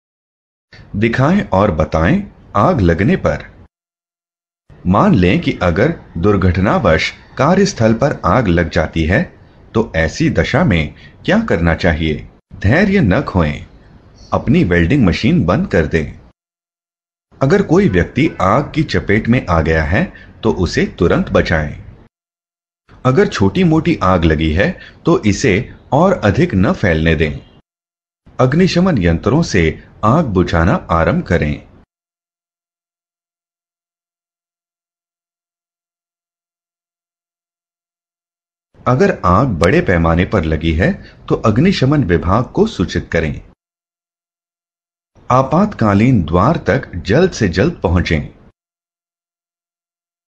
आग लगने पर क्या करें || Things to do if Fire Arises
This video will provide you the list of things to be done in case the fire arises.